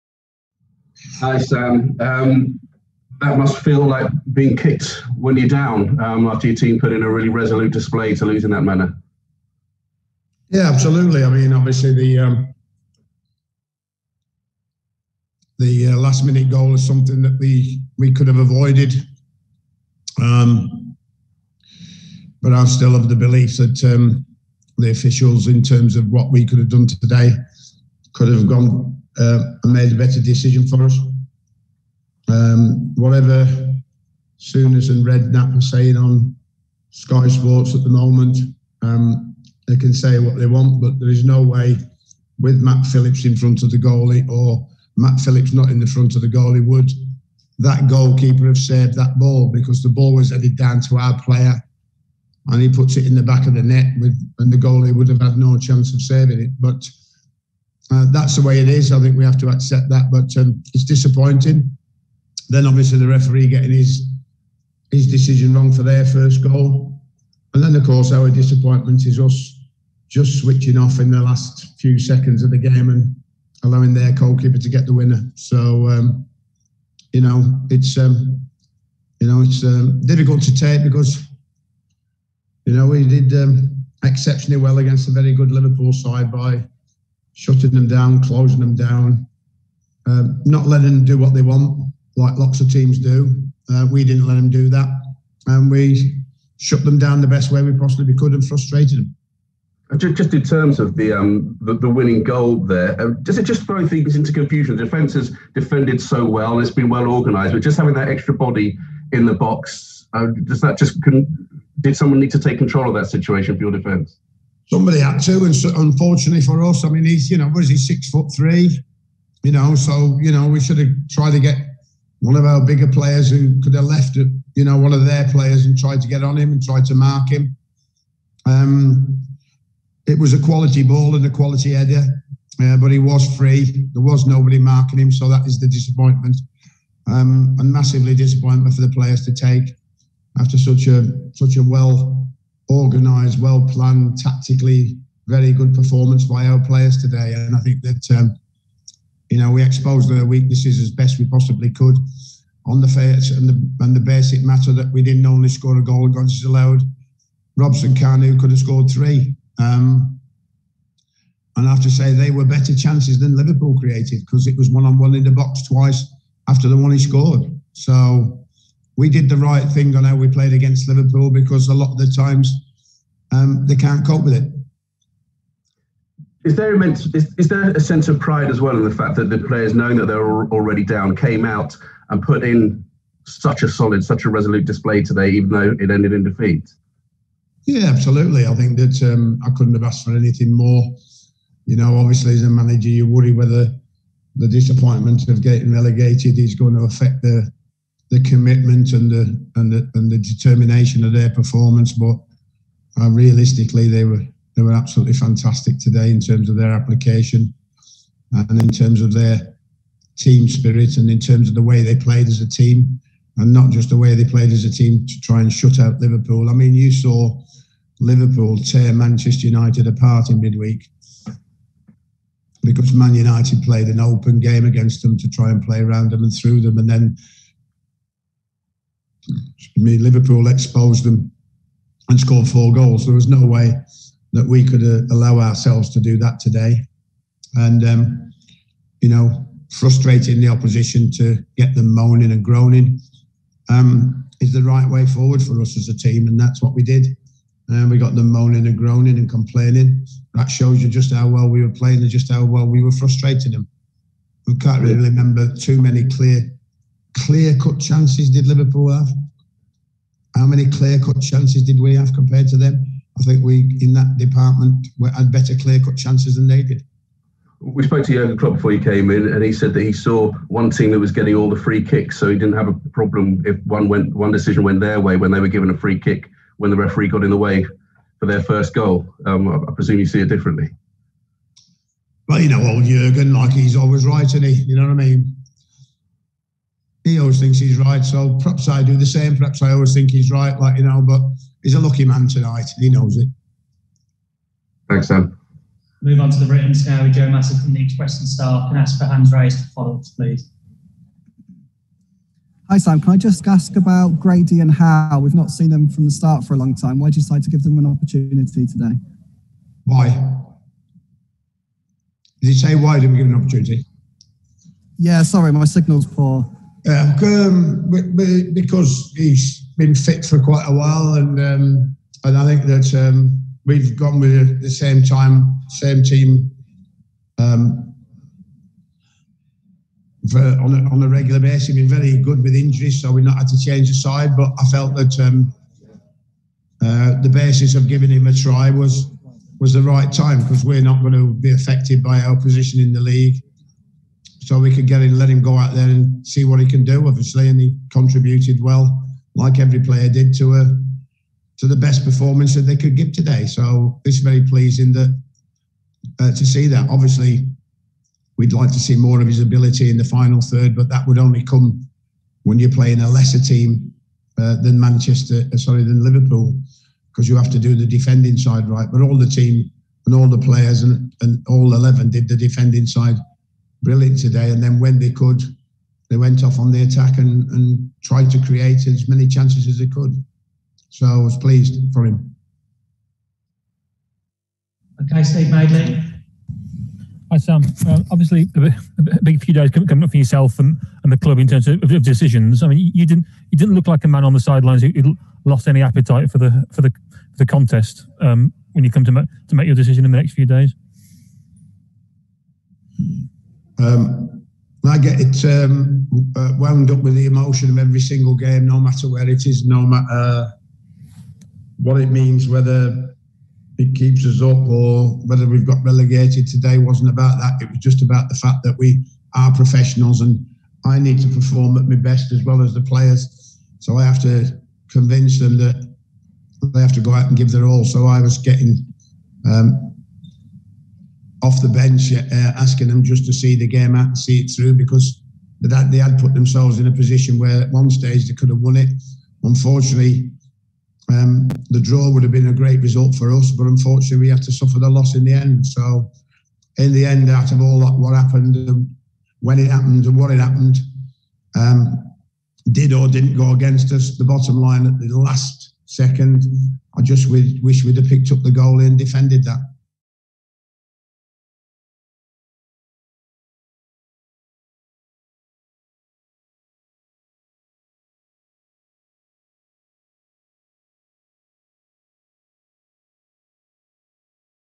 West Brom 1-2 Liverpool - Sam Allardyce - Post-Match Press Conference
West Brom manager Sam Allardyce speaks to the press following his team's 1-2 defeat to Liverpool in the Premier League.